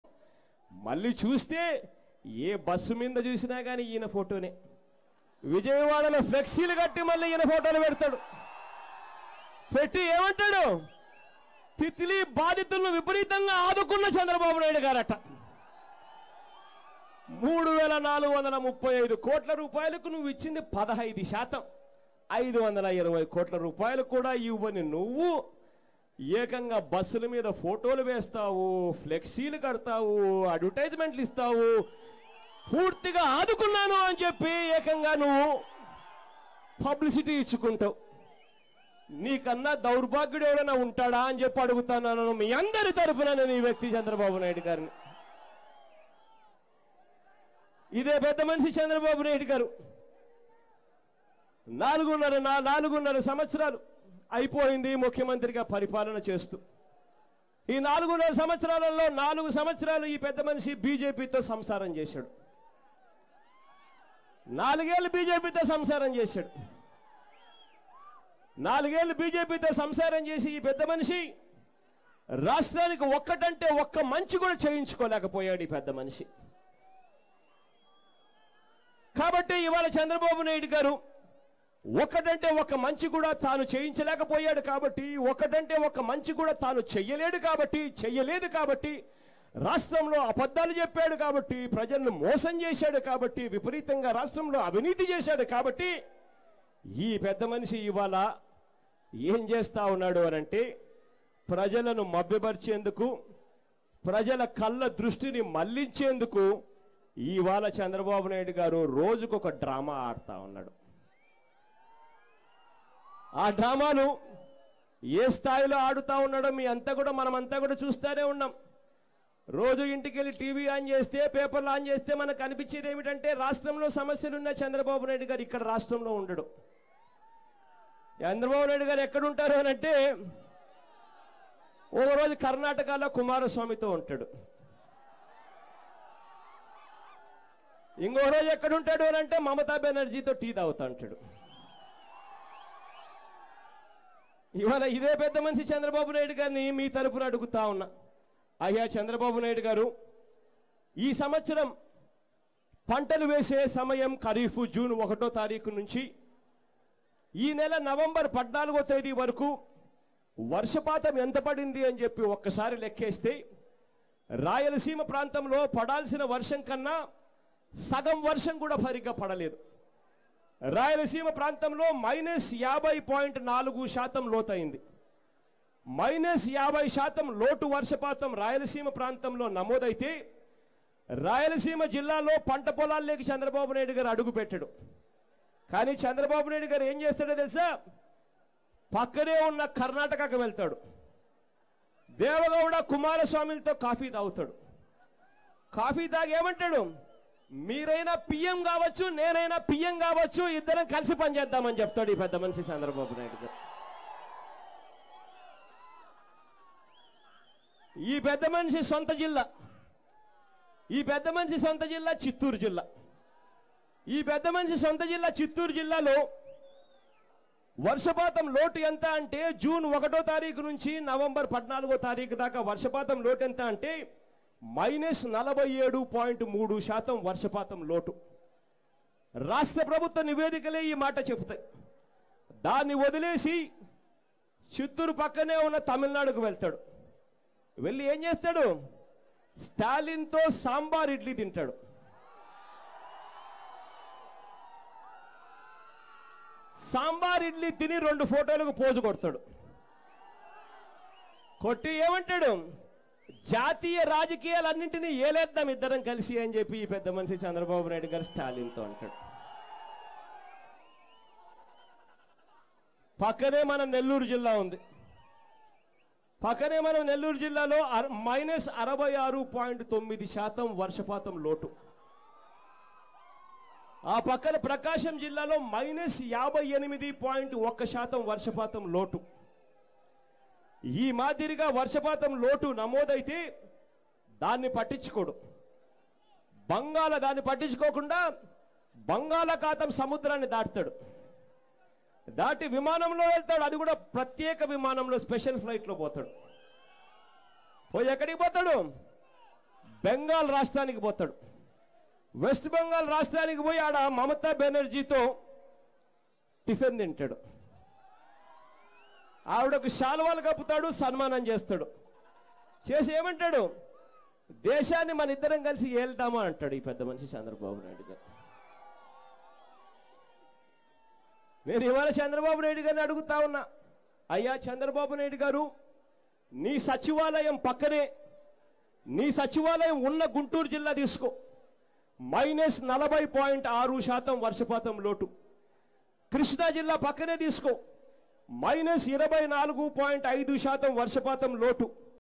YS Jagan Controversial Comments On Chandrababu Naidu Over Meeting With Local Parties | AP24x7
YS Jagan controversial comments on Chandrababu Naidu over meeting with local parties. He slammed Chandrababu Naidu over advertisements in Vijayawada.